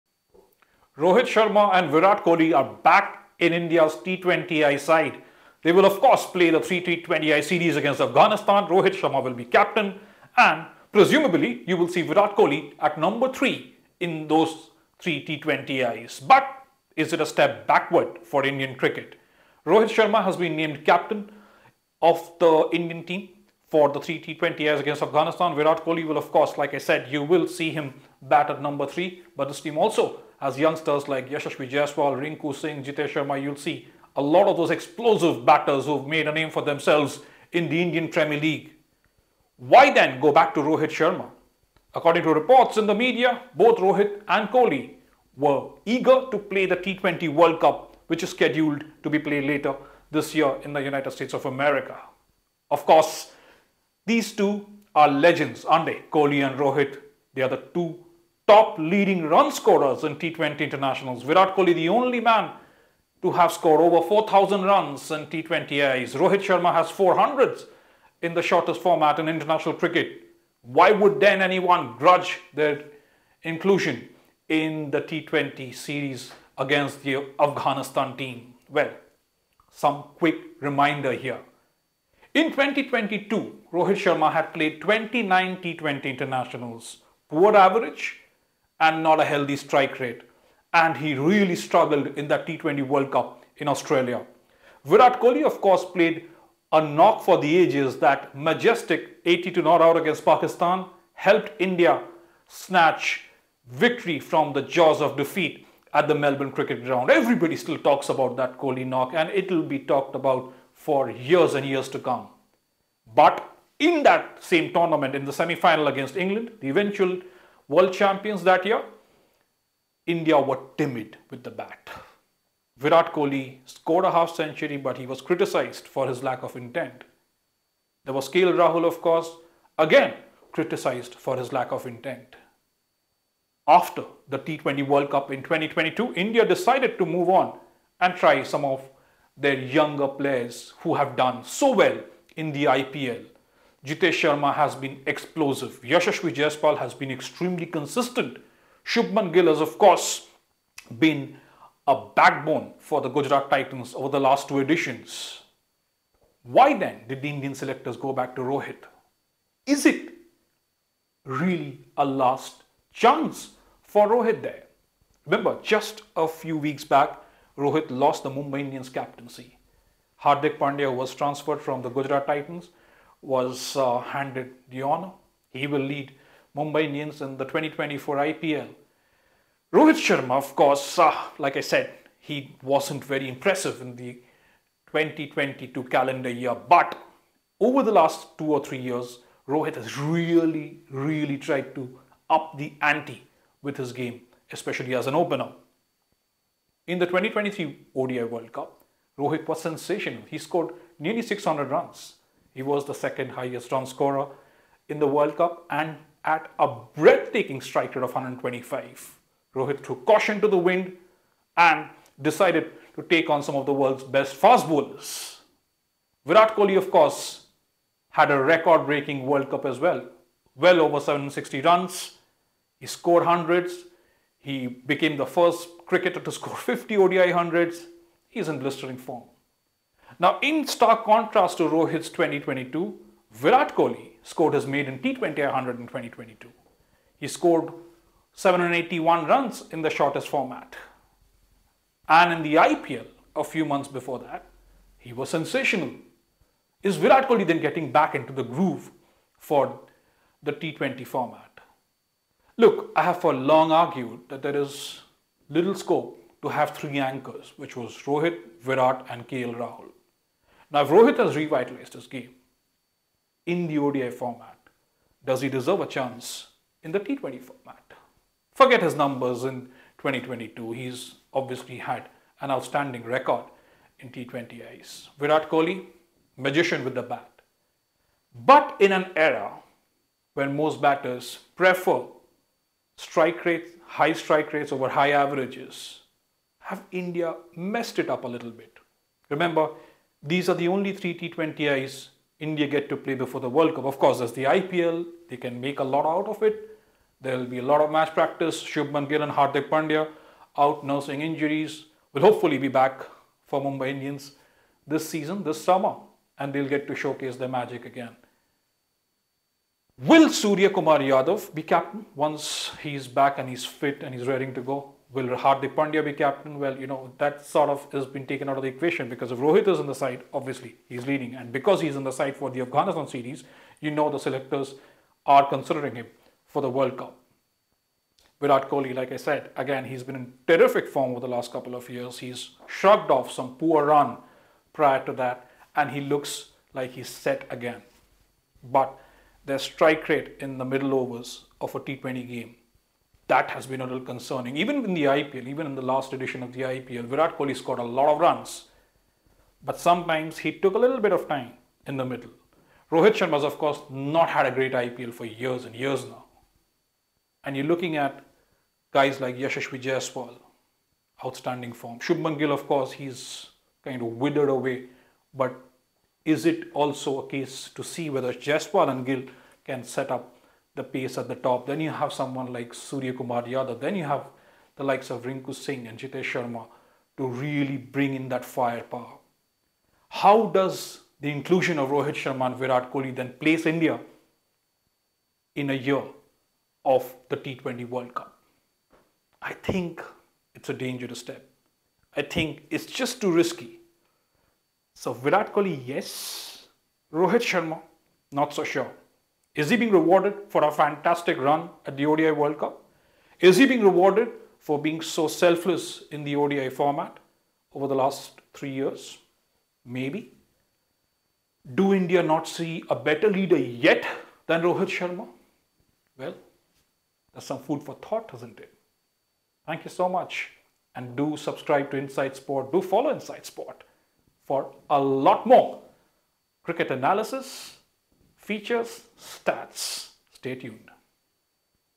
Rohit Sharma, Virat Kohli back in T20I side. Did the Indian cricket team take a backward step?
Rohit Sharma and Virat Kohli were included in the Indian squad for the three-match T20I series against Afghanistan. Rohit, who led India to the final of the 2023 ODI World Cup, returned as captain of the T20I side.
Neither Rohit nor Kohli played a T20 International since the semifinal of the T20 World Cup in November 2022. How does this move  affect the team?
Of course, both men were in outstanding form during the ODI World Cup and they will be eager to show they still got it in the shortest format.
Can Rohit convince the selectors he is their man for the T20 World Cup? Or has he already? Where does this move leave some of the younger batters?
All that and more on InsideSport.